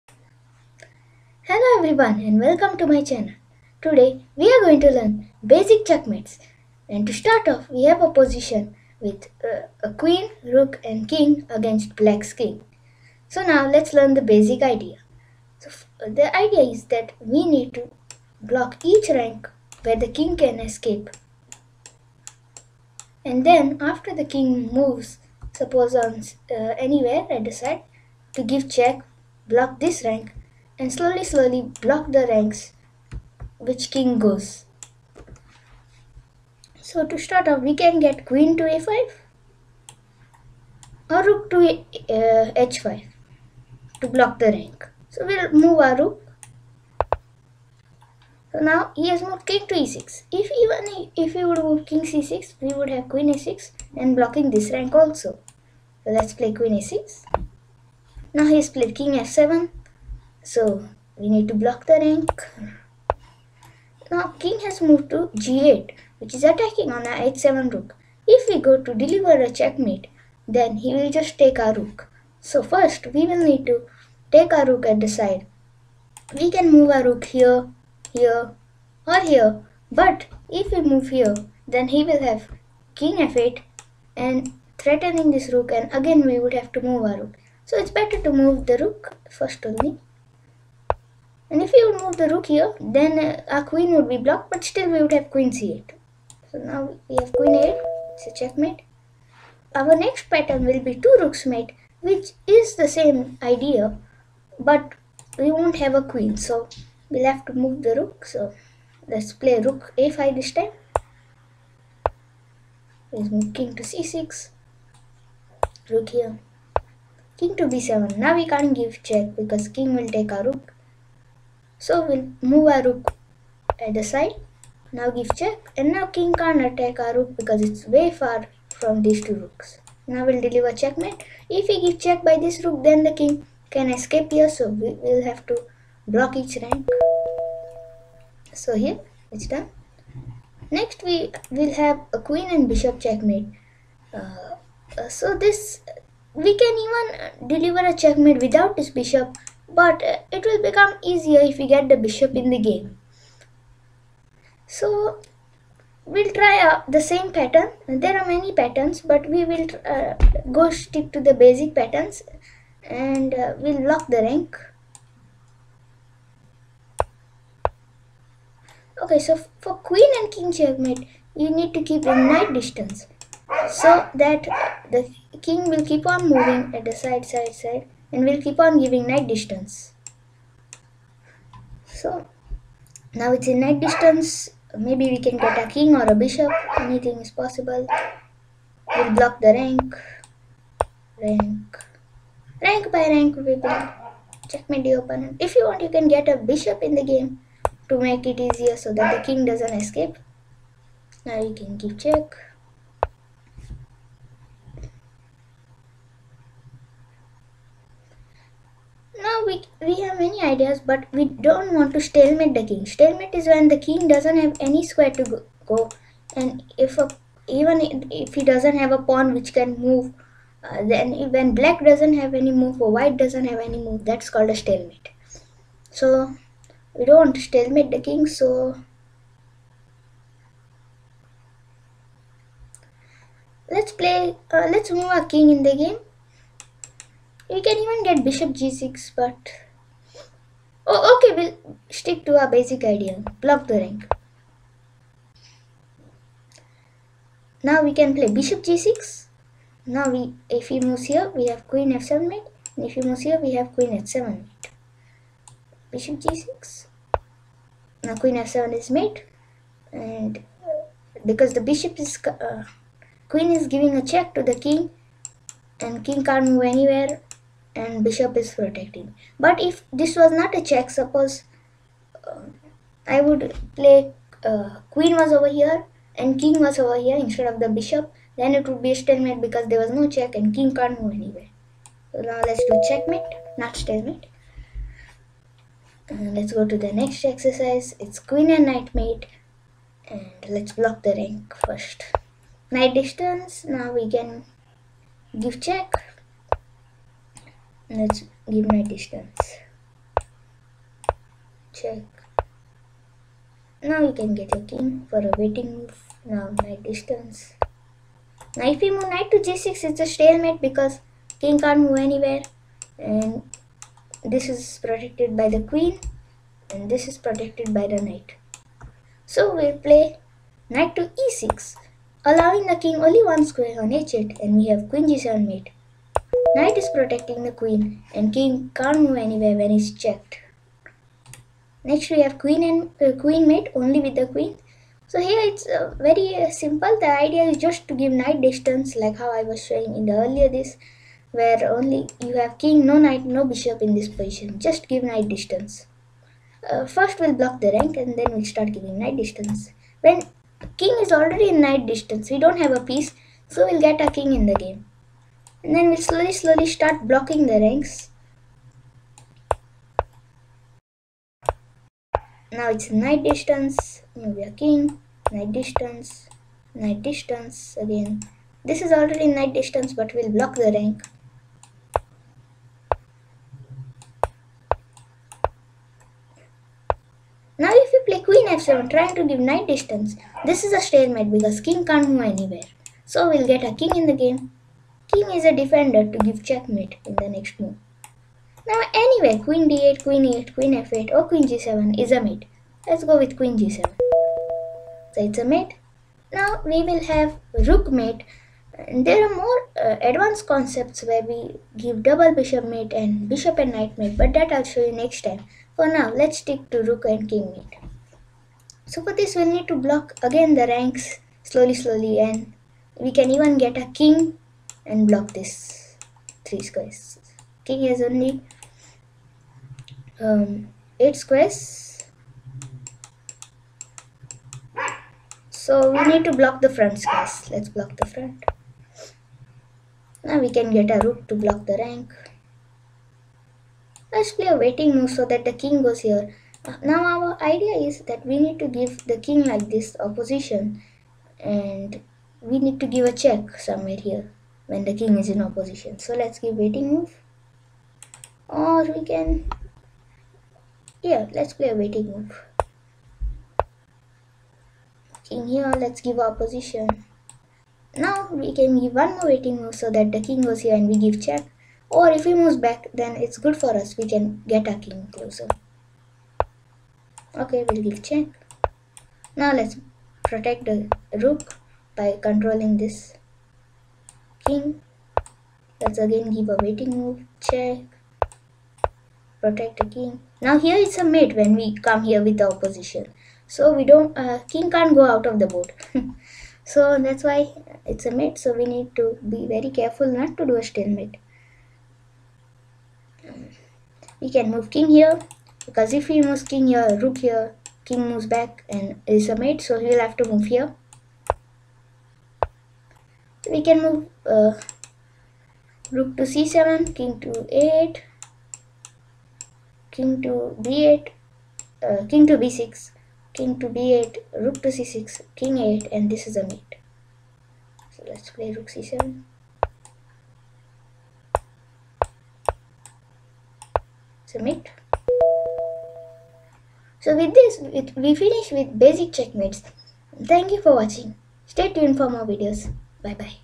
6 Endgame Checkmates YOU MUST KNOW
In this video, AFM Edrick Vaz explains some must know basic chess checkmates to win an endgame.

00:00 Queen & Rook Checkmate
01:39 Double Rook Checkmate
02:20 Queen & Bishop Checkmate
04:45 Queen & Knight Checkmate
05:33 Queen & King Checkmate
06:35 Rook & King Checkmate